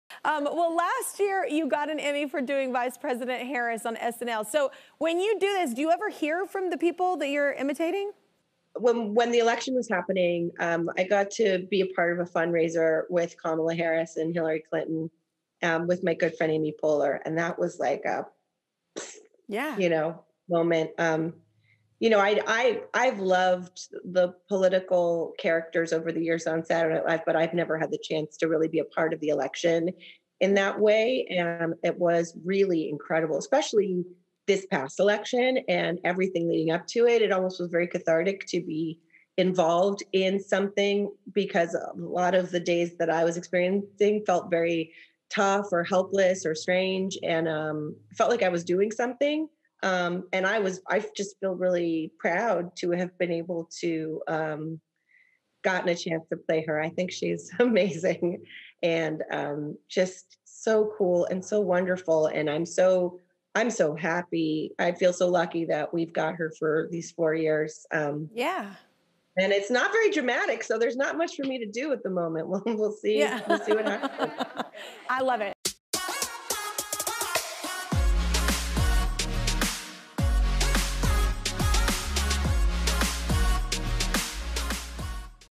Maya Rudolph 'Proud' To Play VP Kamala Harris On 'SNL'
Maya Rudolph won an Emmy last year for playing Vice President Kamala Harris on 'SNL,' her first political character on the show during an election year. Maya says she was "proud" to play the role, and reveals that she had the opportunity to meet VP Harris and Hillary Clinton at a fundraiser along with fellow 'SNL' alum Amy Poehler.